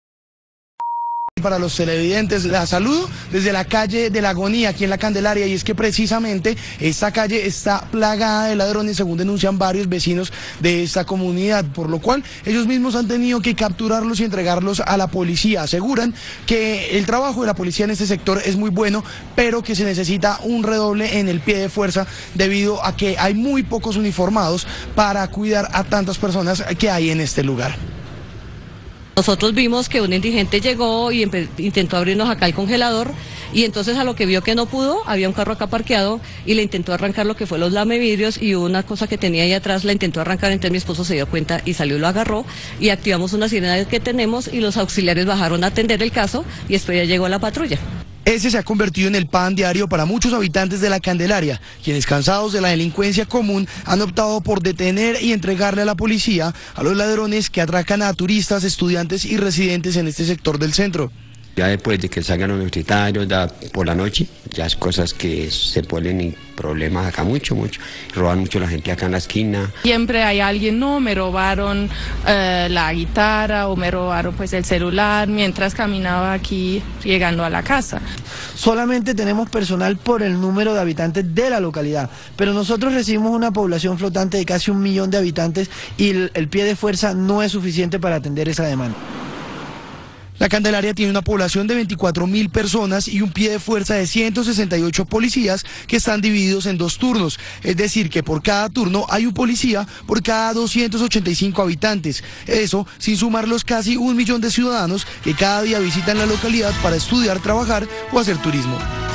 Habitantes de la candelaria aburridos con la inseguridad del sector| City TV | Enero 26
Vecinos y comerciantes de la candelaria están aburridos de los atracos callejeros.